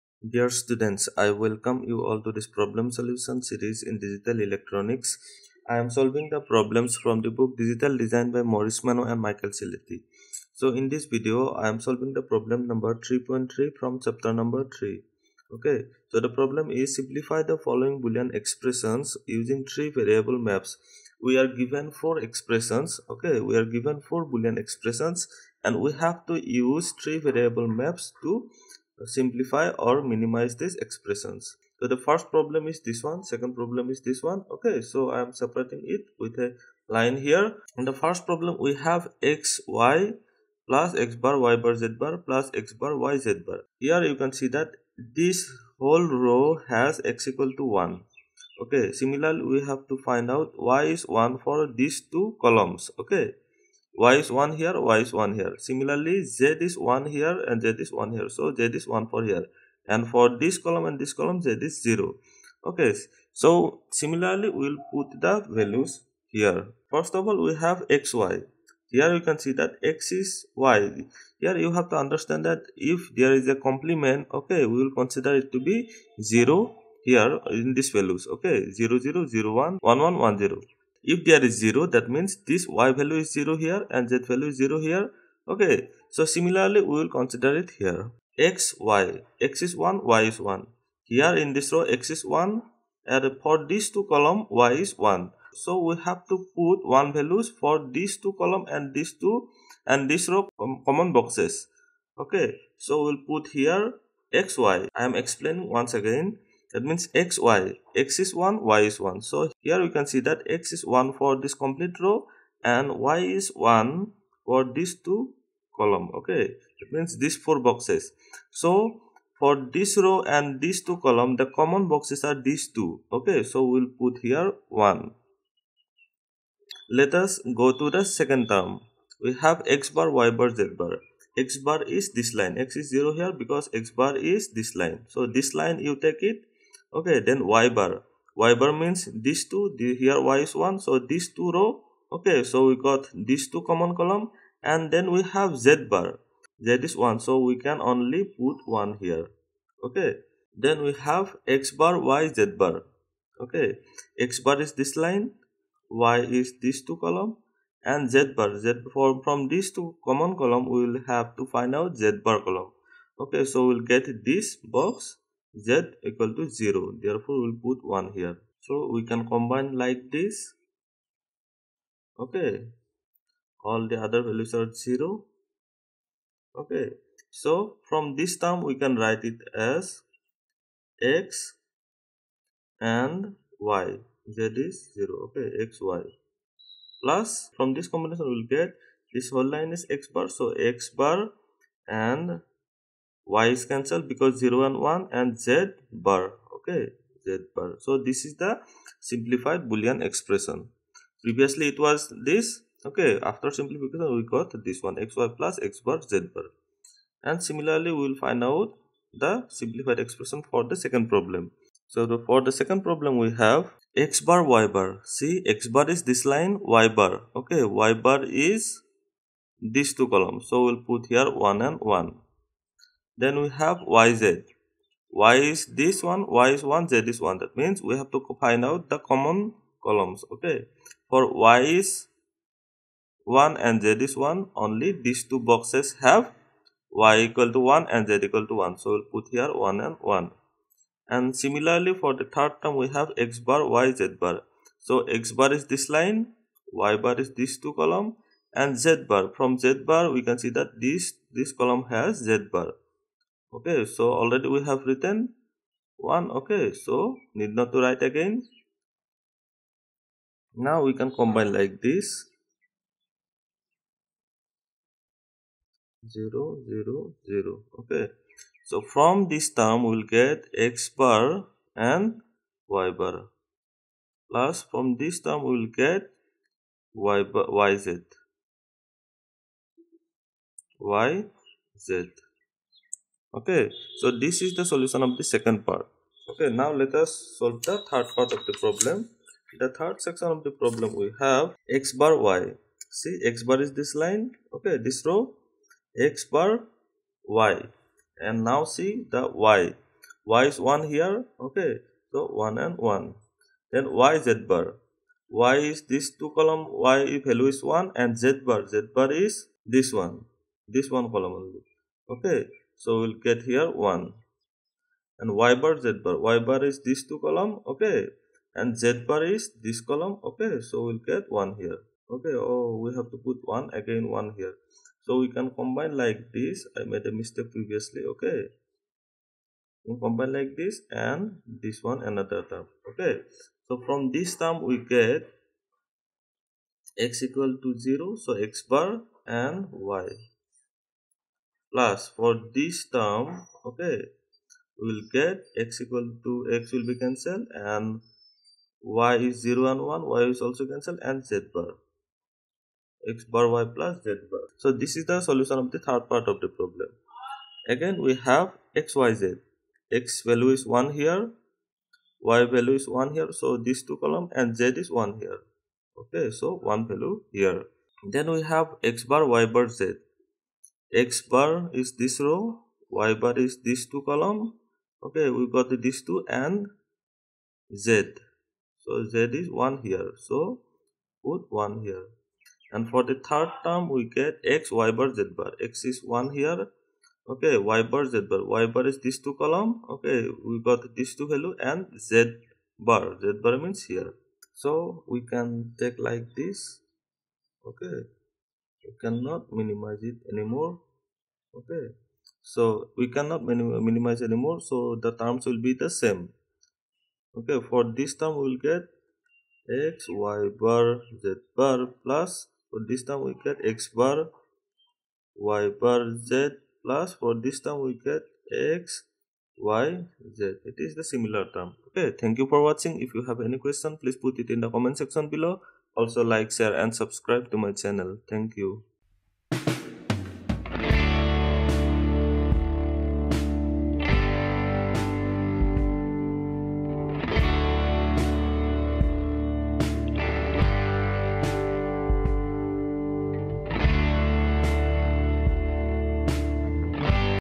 Q. 3.3: Simplify following Boolean expressions, using three-variable maps: (a) xy + x'y'z' + x'yz'
For part (b), I forgot to put x'yz' in the k-map. So after adding, the correct answer will be x'+yz

Q. 3.3: Simplify the following Boolean expressions, using three-variable maps:
(a) xy + x'y'z' + x'yz'
(b) F(x,y,z) = x'y' + yz + x'yz'
(d) F(x,y,z) = xyz + x'y'z + xy'z'